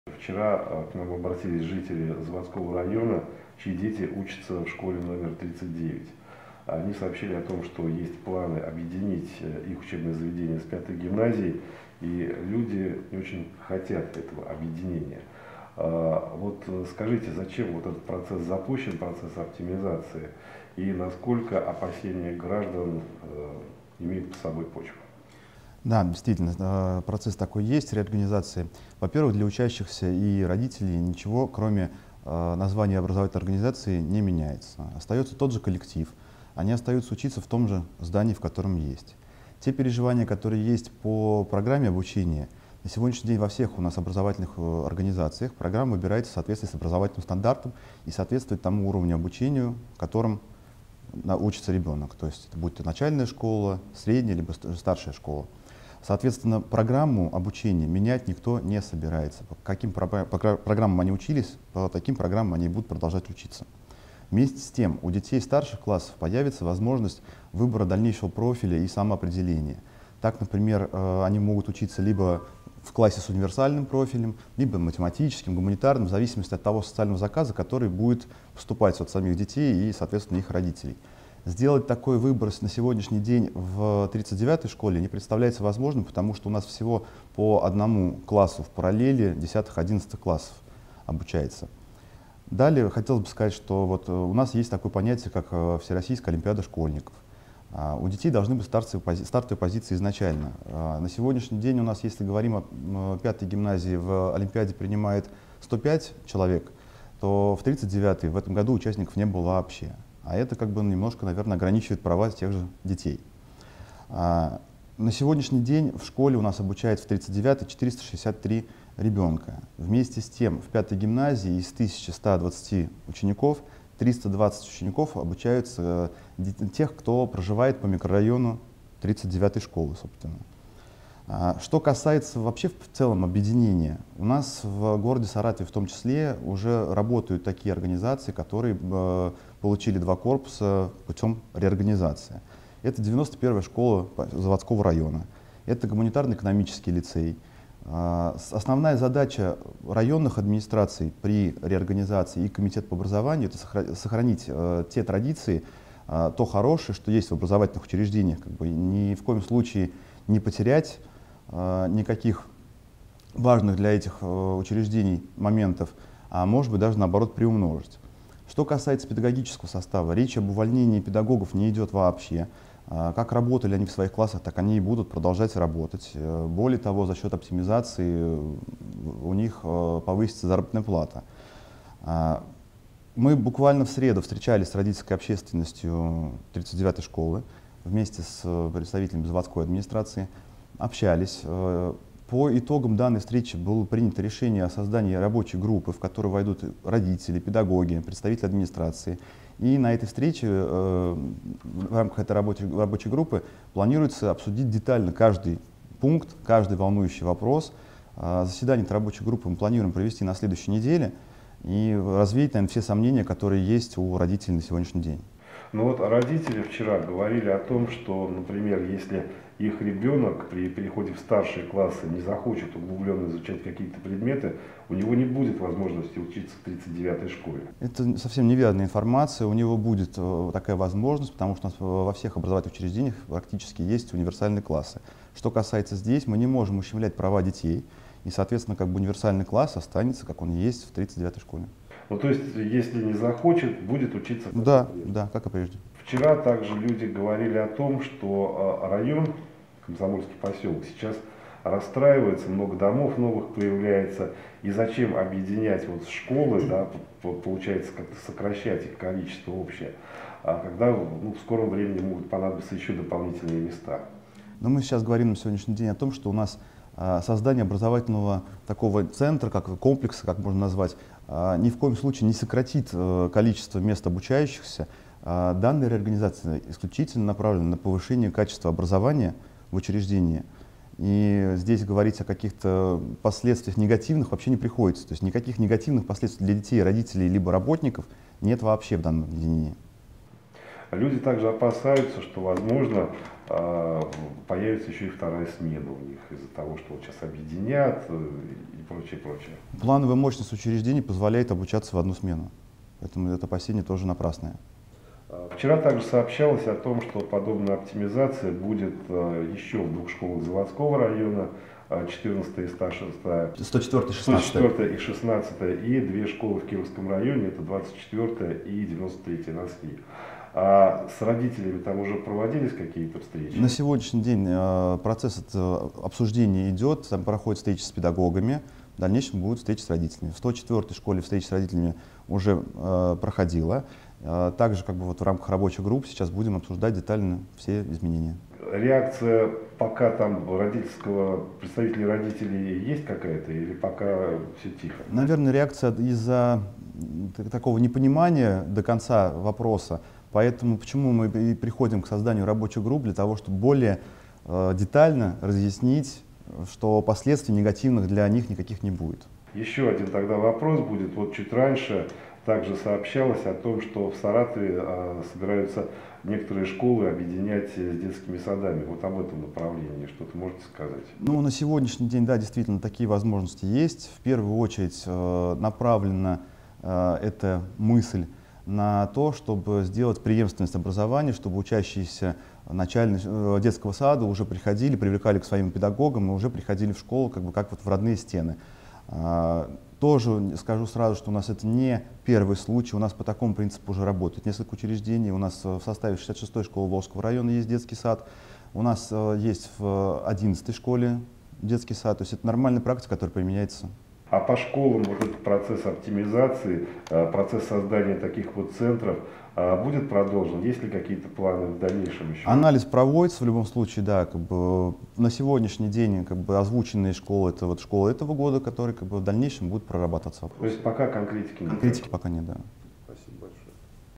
Оптимизация школ в Саратове. Вопросы родителей и ответы Александра Блатмана
Оптимизация 39-й саратовской школы и гимназии №5 пойдет на пользу учащихся. Такое мнение в интервью ИА "Взгляд-инфо" высказал председатель городского комитета по образованию Александр Блатман.

Накануне родители учеников МОУ "СОШ" №39 выступили с видеообращением к мэру Михаилу Исаеву.

Граждане сообщили градоначальнику, что категорически не поддерживают инициативу чиновников объединить их учебное заведение с гимназией.

В качестве главного аргумента они называли различие программ, по которым проходят обучение школьники и гимназисты.